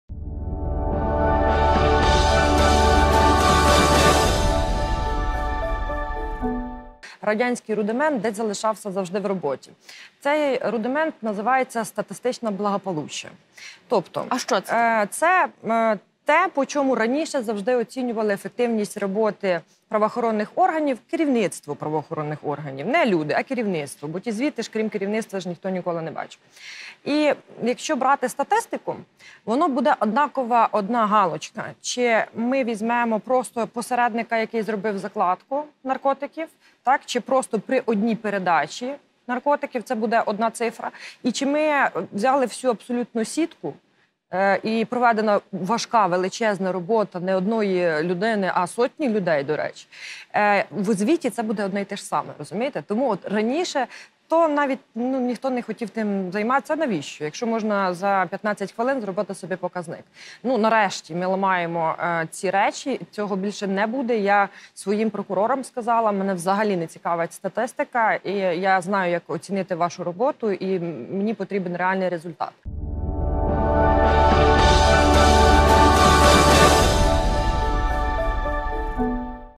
Прокурорка Львівщини Ірина Діденко розповіла про застарілі методи оцінки роботи прокурорів
«Навіщо викривати масштабну схему, якщо за 15 хвилин можна зробити собі показник⁉️» - Прокурорка Львівщини Ірина Діденко розповіла про застарілі методи оцінки роботи прокурорів